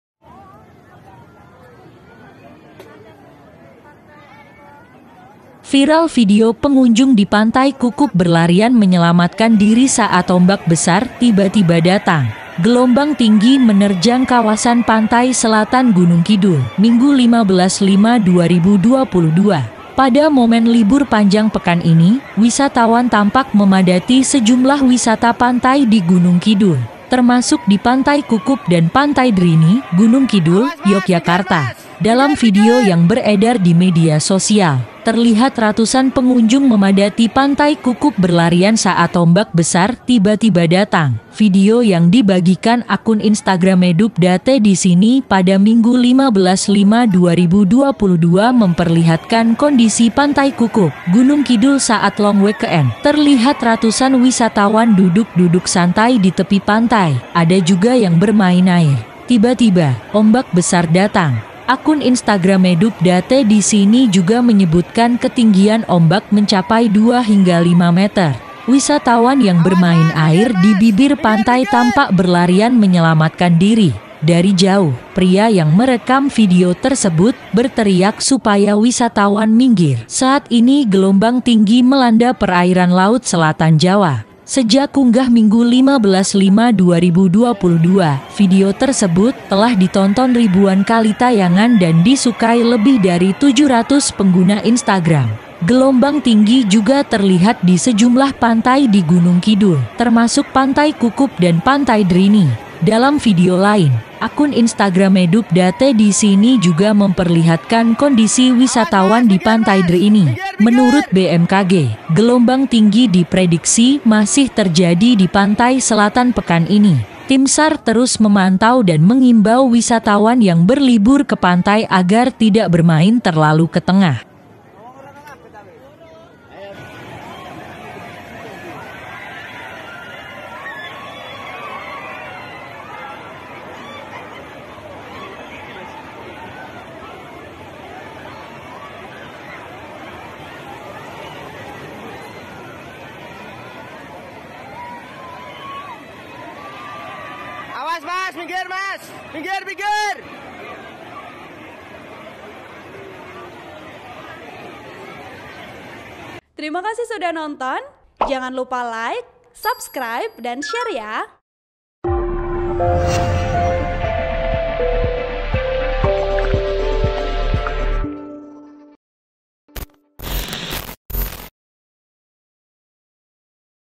Viral Ratusan Pengunjung Pantai Gunungkidul Berlarian Menyelamatkan Diri saat Ombak Tinggi Datang
TRIBUNTRAVEL.COM - Viral video pengunjung di Pantai Kukup berlarian menyelamatkan diri saat ombak besar tiba-tiba datang.

Gelombang tinggi menerjang kawasan Pantai Selatan Gunungkidul, Minggu (15/5/2022).

Editor Rheymeldi
Sumber IG/ updatedisini